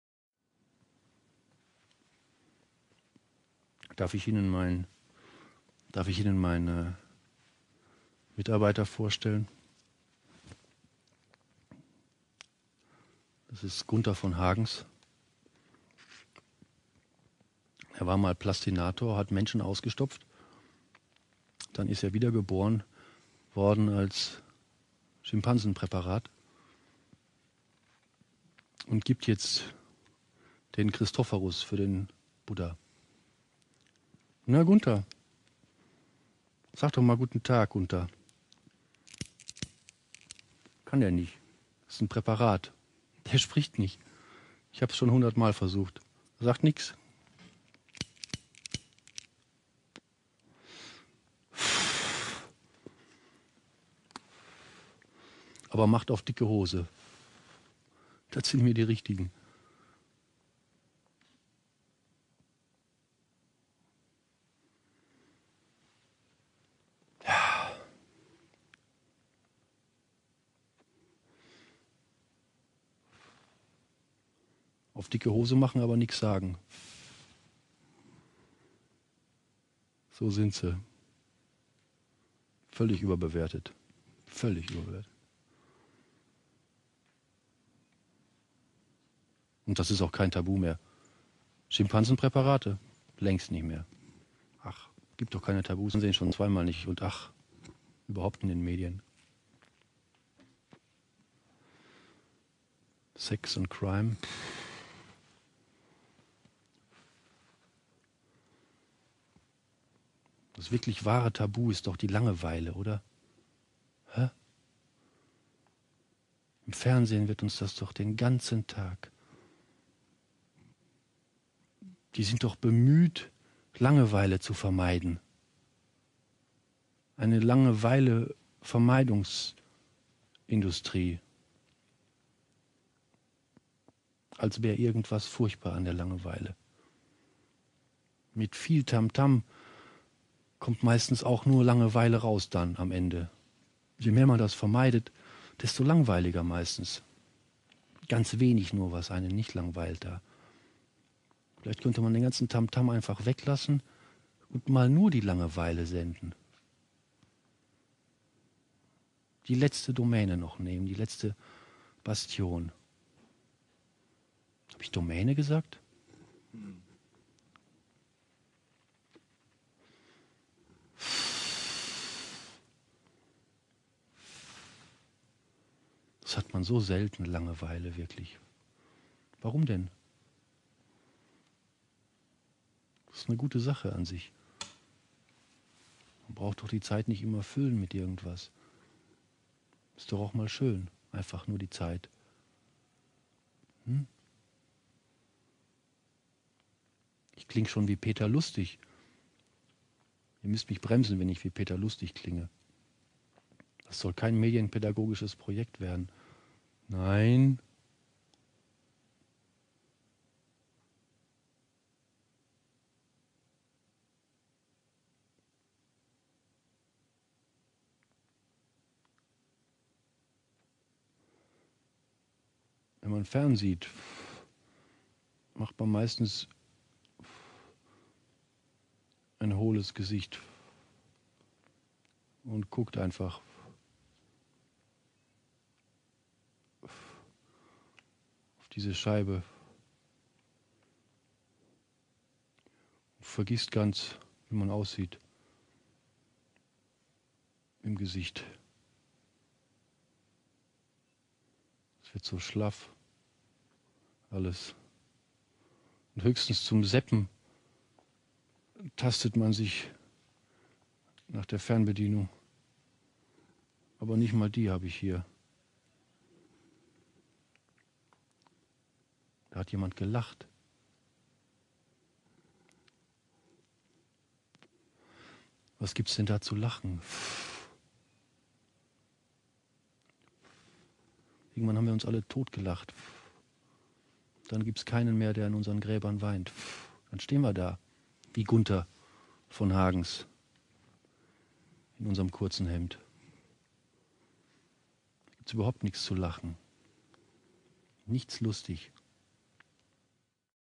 Keine Tabus
Hagen Rether

spricht wahres ruhig und gelassen aus...